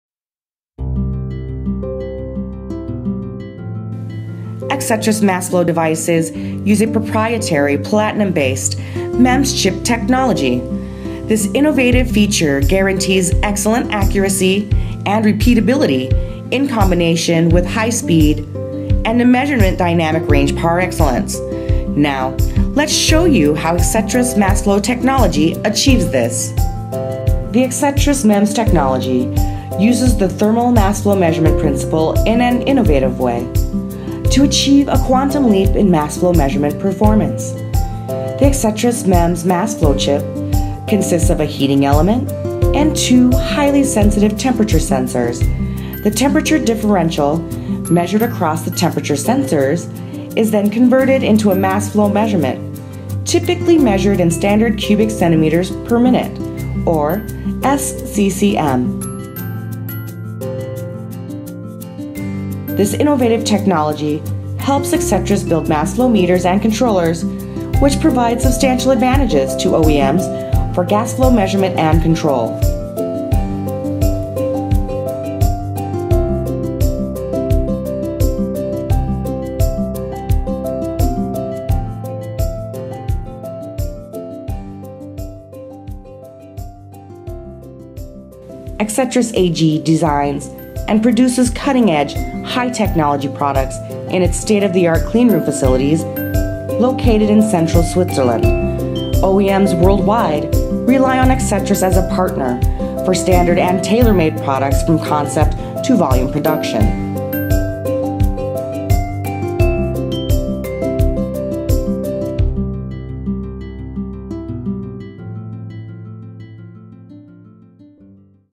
MEMS Mass Flow Technology by Axetris - Advantages for Customers
Axetris uses the thermal mass flow measurement principle in an innovative way to build high-performance mass flow devices, bringing concrete advantages for customers such as the highest accuracy and repeatability, quick response time, unmatched dynamic range, and a very compact size!

The Axetris mass flow technology is used by many leading companies in the field of gas chromatography, leak testing, thermo analytics, mass spectroscopy, thin film deposition, plasma engineering and more.